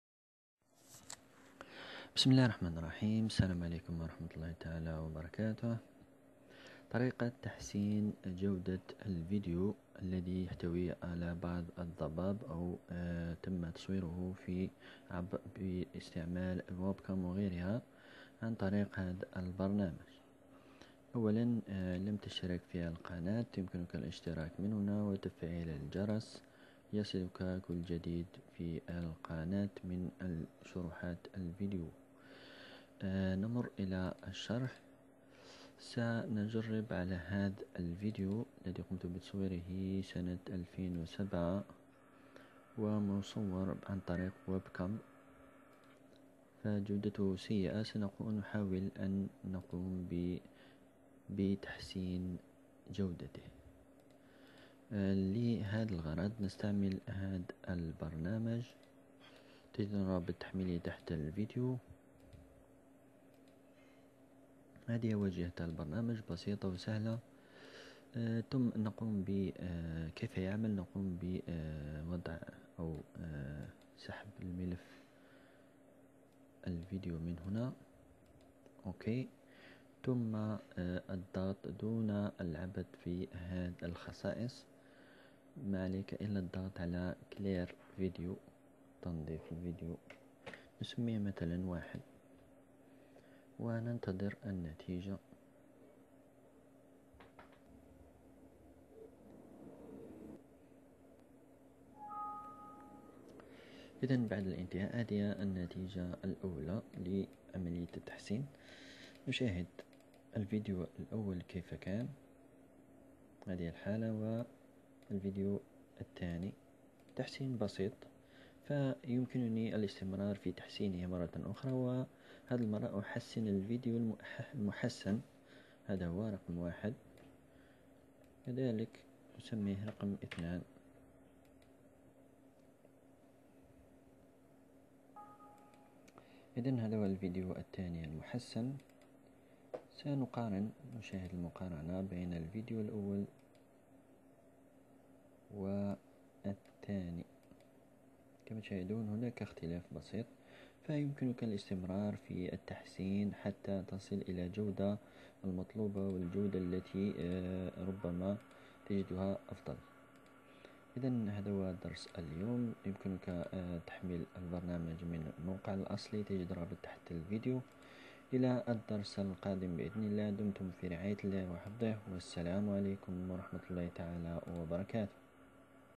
تحسين جودة مقاطع الفيديو الرديئة و الضبابية
شرح بسيط و سريع لطريقة تحسين نوعية او تقليل الضبابية في الفيديوهات القديمة المصورة ربما عن طريق واب كام .. برنامج سهل الاسخدام يمكنك تحسين الفيديو و تكرار العملية الا ان تحصل على احسن جودة تعجبك .. للغشارة هده الخدمة موجود في نظام اليوتوب عندما تقوم بنشر فيديو بنوعية رديئة فتلقائيا يقوم باقتراح عليك تحسين الفيديو الى جودة افضل ...
البرنامج : Blurry Video Clearer Free